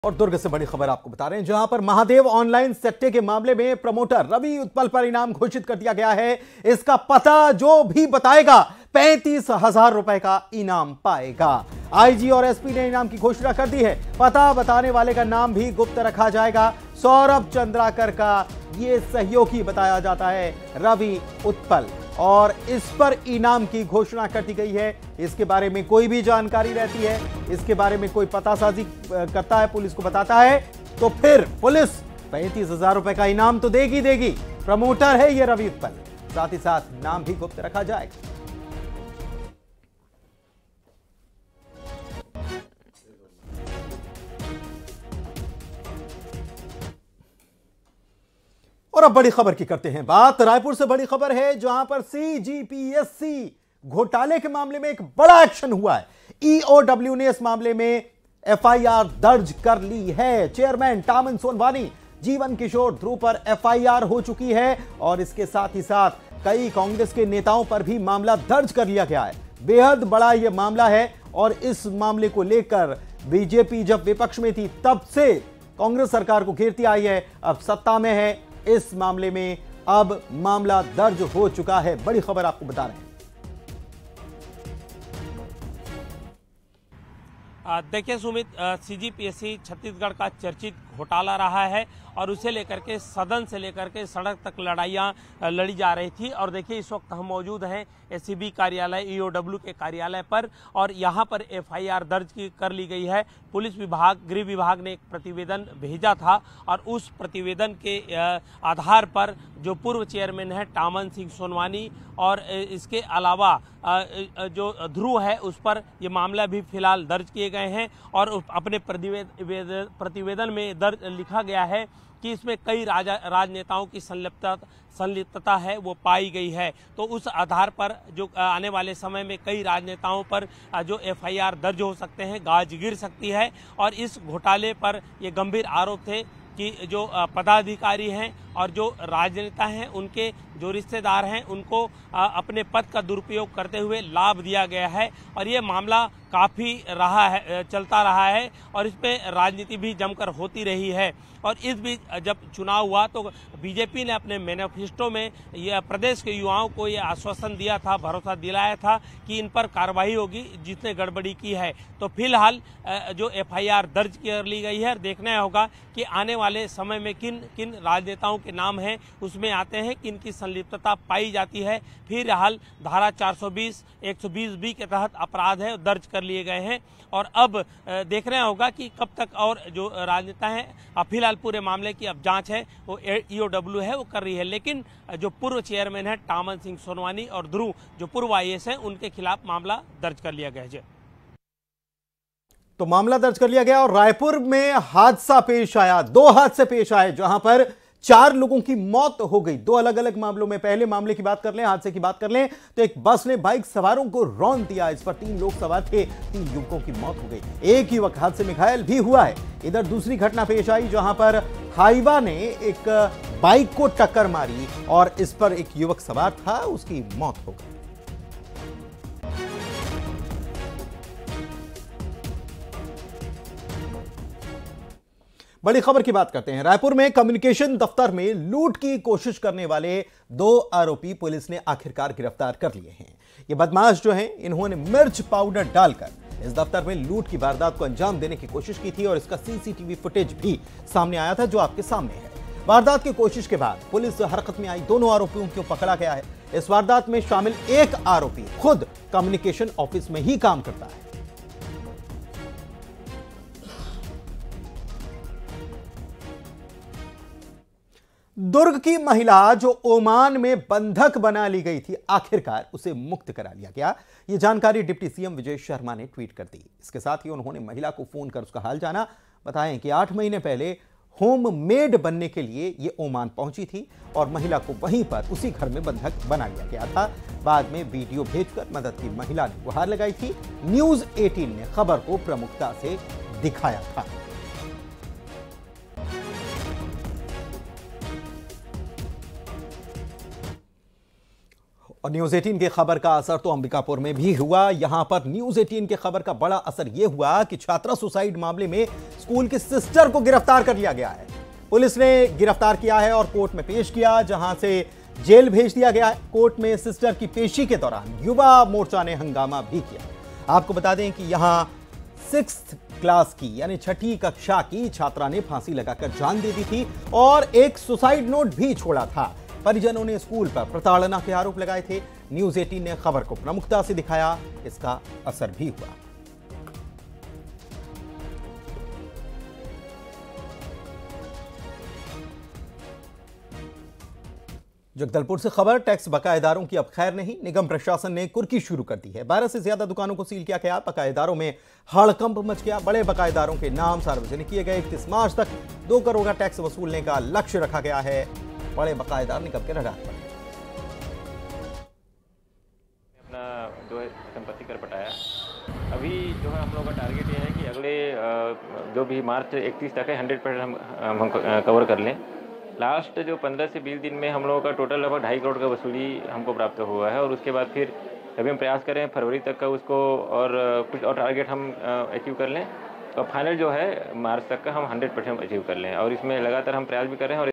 Mahadev Betting App Update : Mahadev Betting App के Promoter Ravi Utpal पर इनाम घोषित |Breaking News
Mahadev Betting App के Promoter Ravi Utpal पर इनाम घोषित, इनाम बताने वाले को 35 हजार रुपये दिए जाएंगे ..